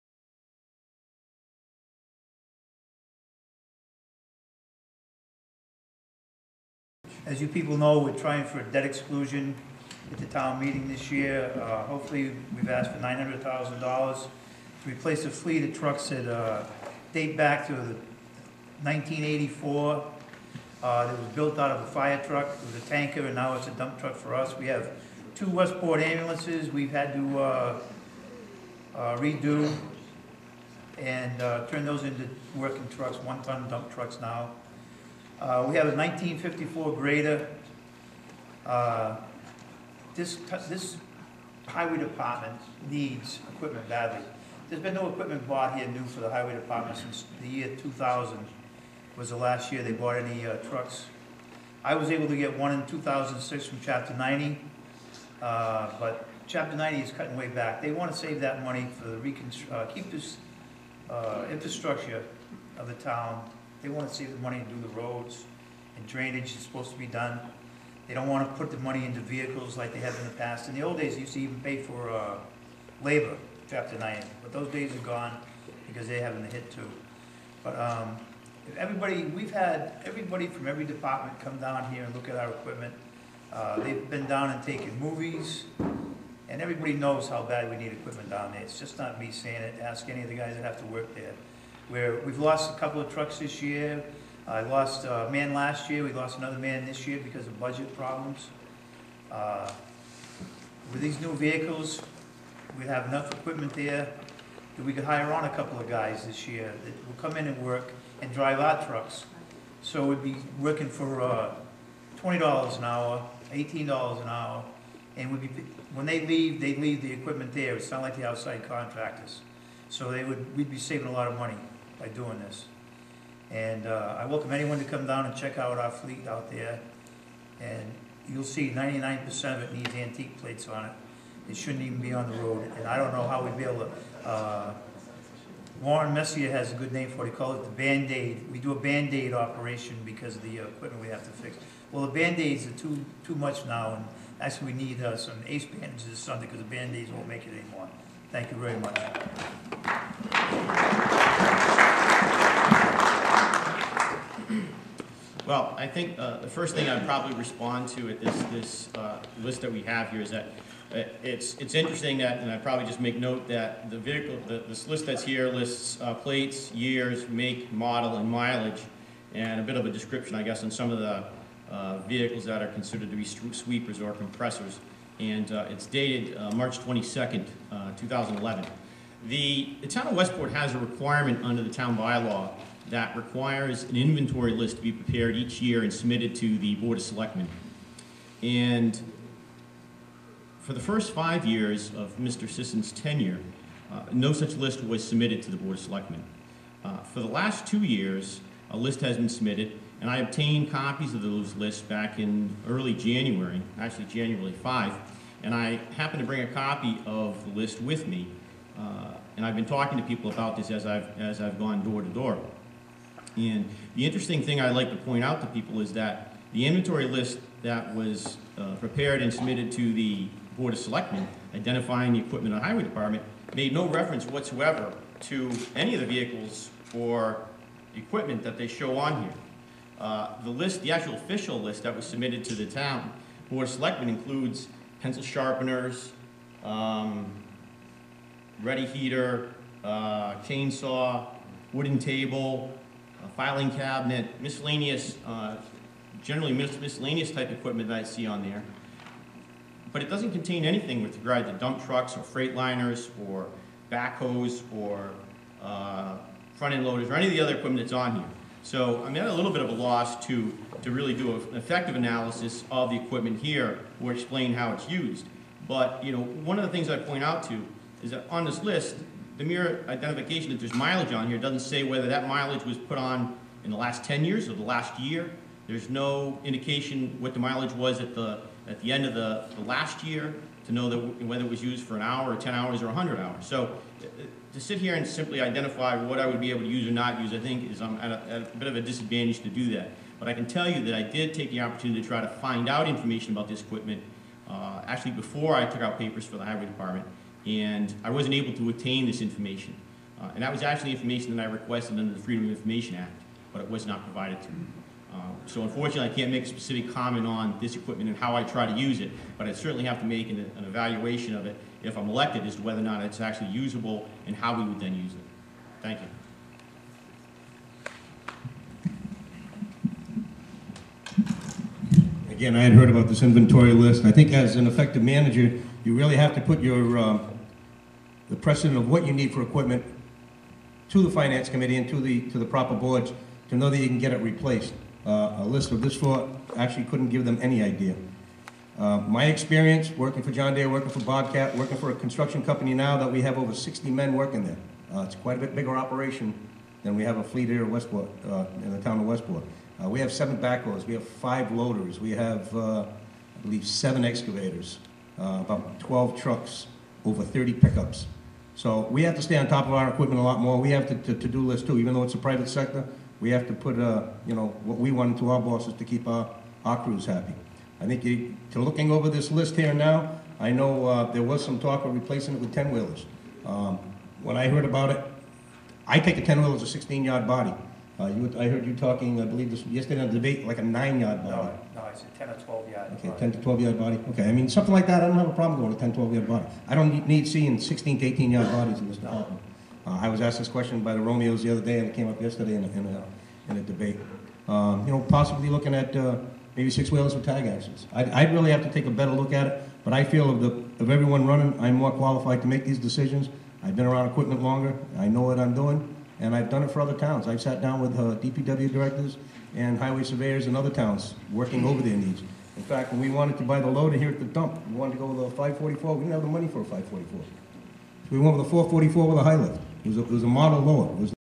Westport Highway Surveyour candidates speak.
Westport Highway Surveyour candidates speak on equipment inventory list at Candidates' Night = March 30, 2011.